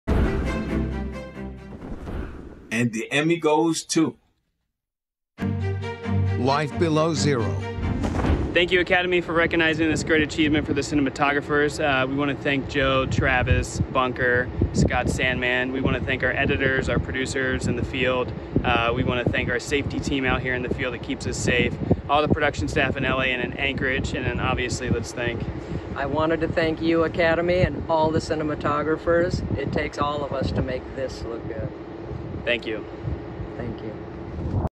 2020 Creative Arts Emmys: Cinematography for a Reality Program
The team from National Geographic wins the Emmy for Outstanding Cinematography for a Reality Program.